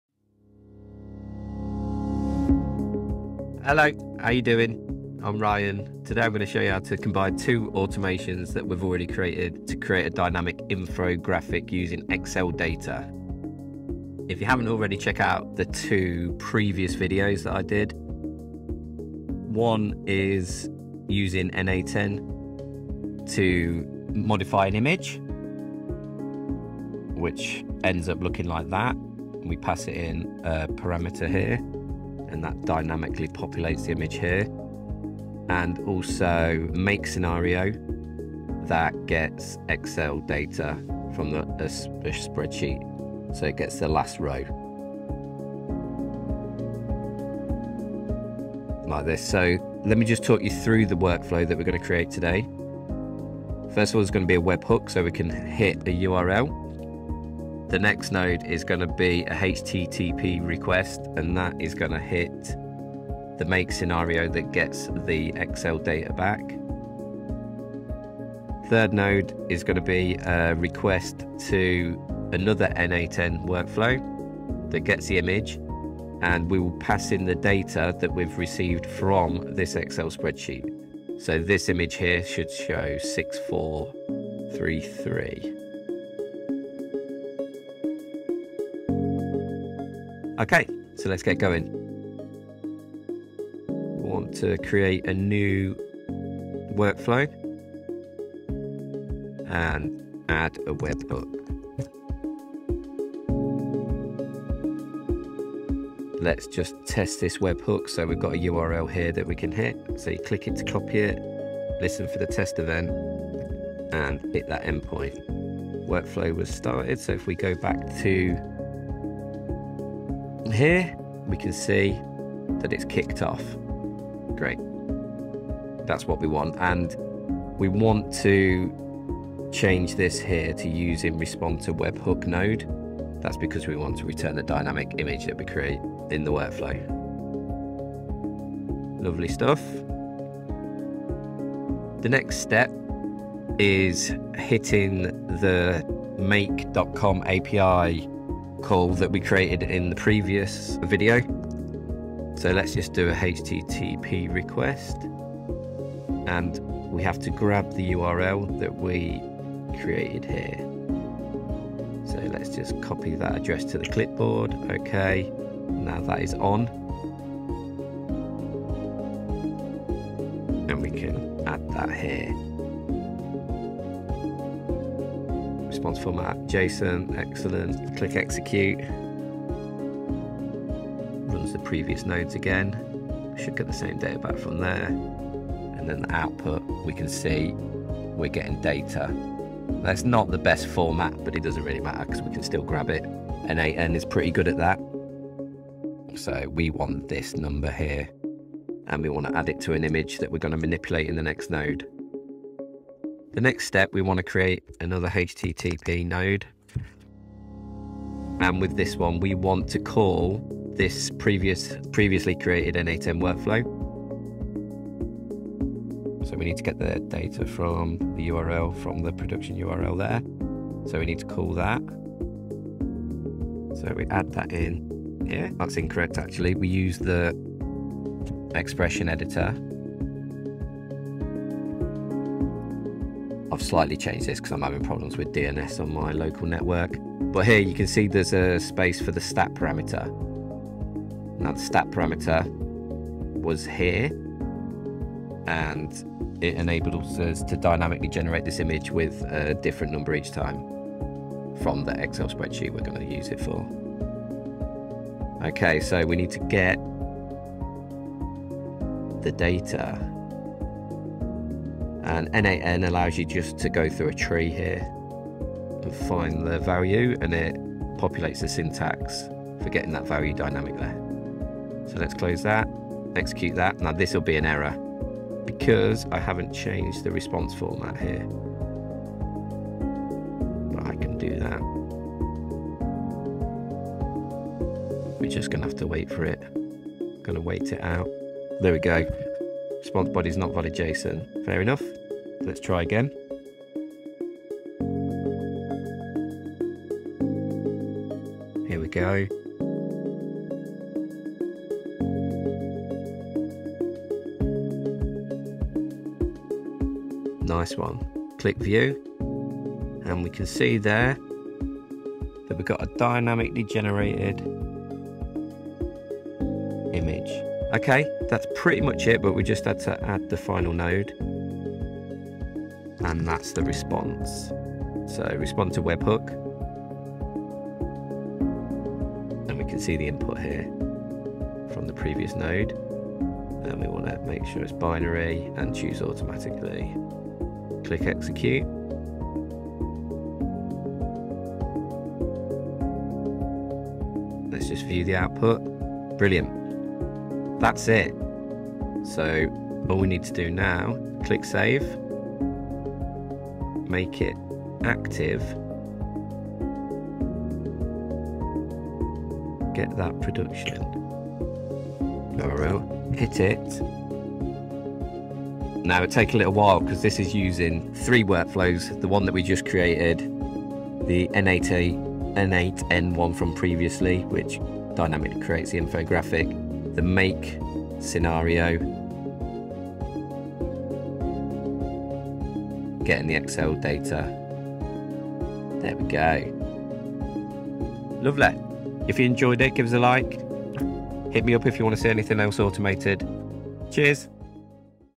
Learn how to dynamically create an infographic with Excel data
Combine our previous 2 video tutorials using n8n, Make and Excel to create a powerful workflow automation that you can use to dynamically generate images containing data from your spreadsheets.